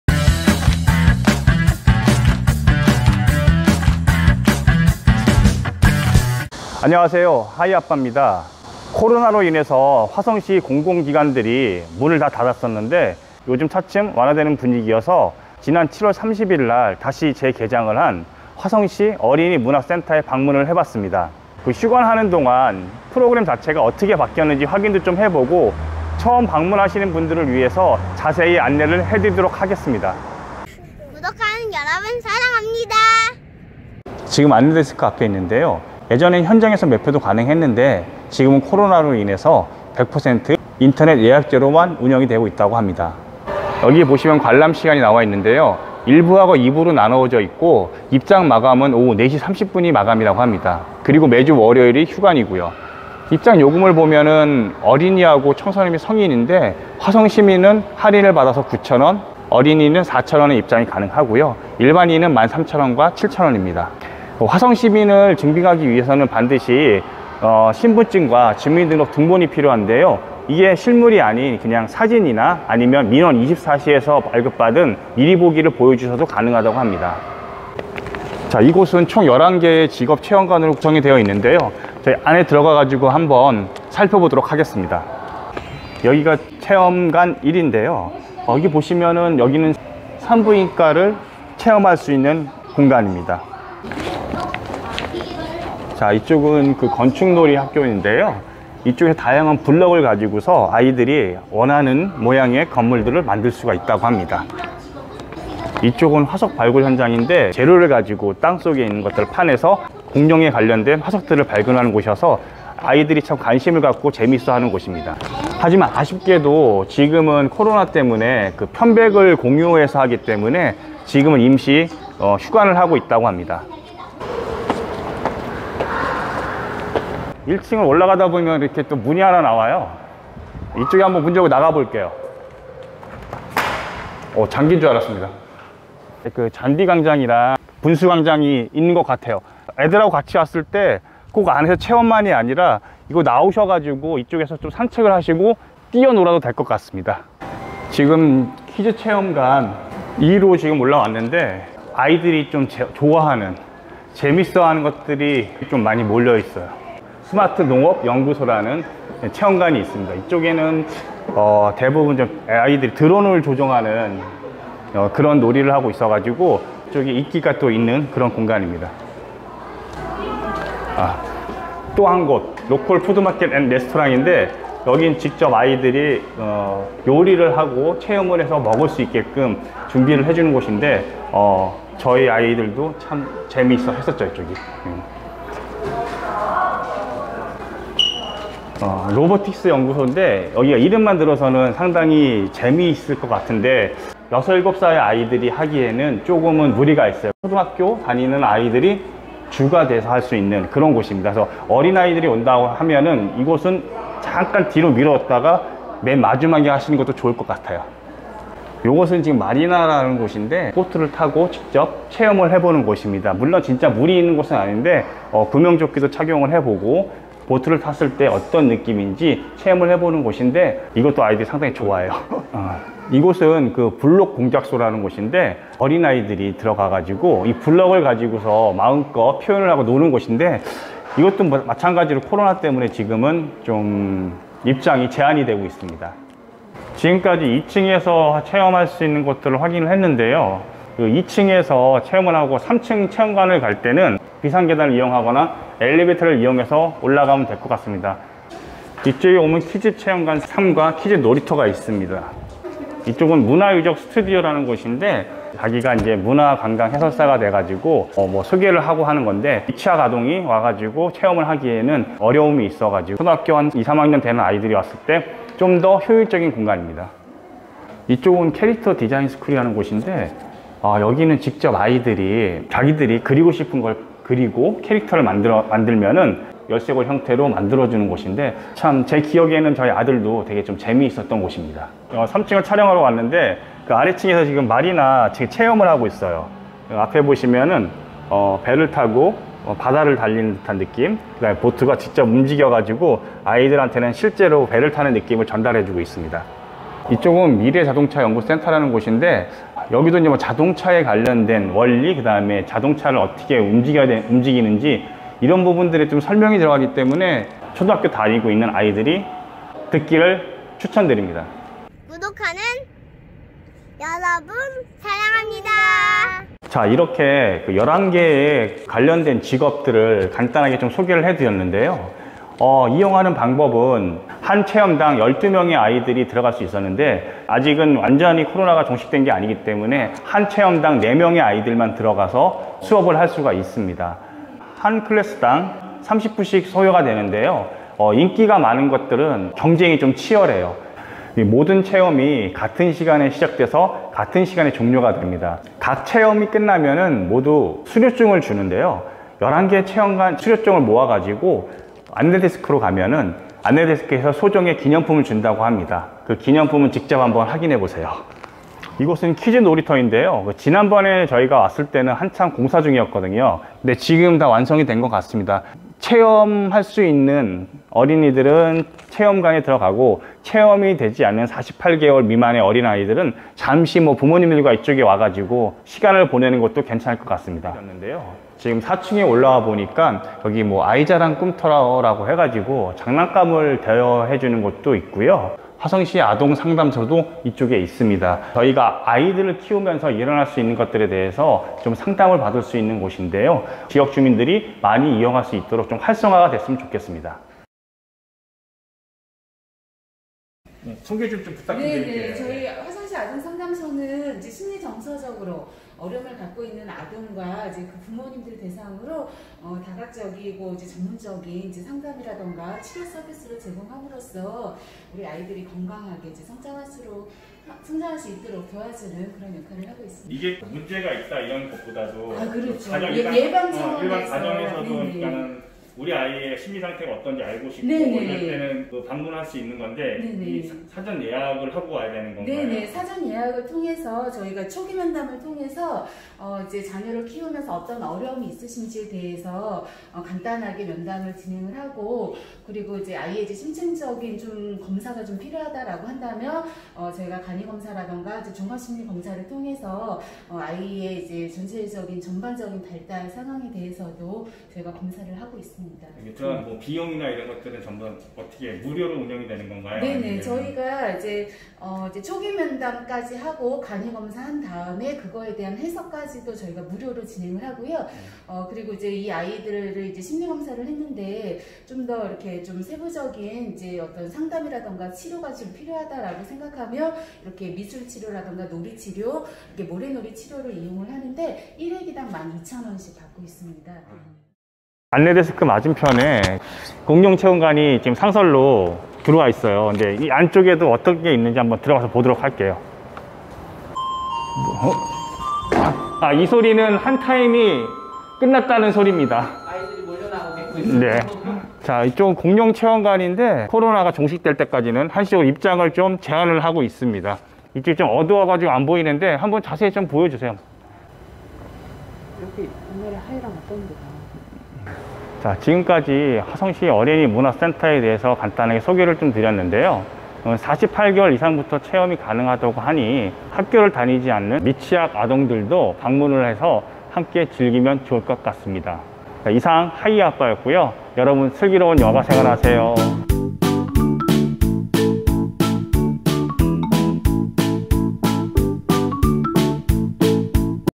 [2020] 어린이 직업체험이 가능한 "화성시어린이문화센터" 방문기
2019년 에 개장한 "화성시어린이문화센터" 를 방문해봤습니다
어린이직업체험 을 비롯 다양한 시설들이 갖춰져 있어 아이들과 함께
방문하여 즐거운 시간을  보내실수 있는공간입니다
하이아빠와 미리 다녀와보시고 아이들과 즐거운 시간들 보내세요